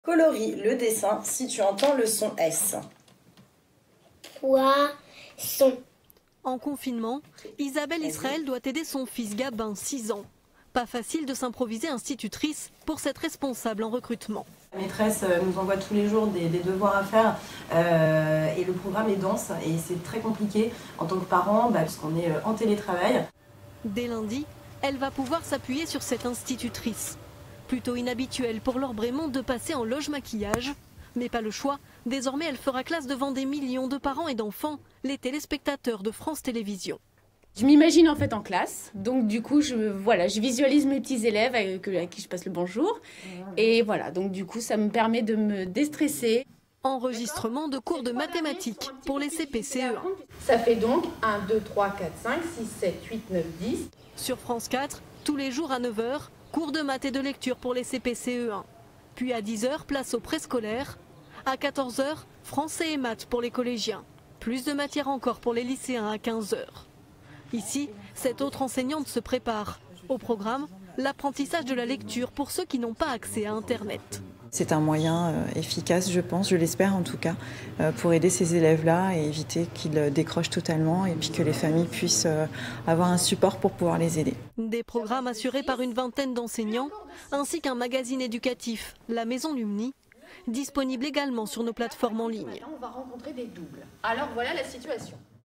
Education, engagement de l’audiovisuel public
Pour accompagner les enfants, dès ce lundi 23 mars 2020, France 4 bouleverse totalement sa grille pour diffuser en direct des cours dispensés par des professeurs de l’Education nationale... Il y en aura pour tous les niveaux... scolaires... L'audiovisuel public se mobilise à la télévision, mais aussi sur internet. Reportage de Sophie Lanson, Liane Courté, Mickael Cario et S.Lacombe avec Isabelle Israël (mère d'élève), Laure Brémont (enseignante) Christine Pereira (enseignante de CP)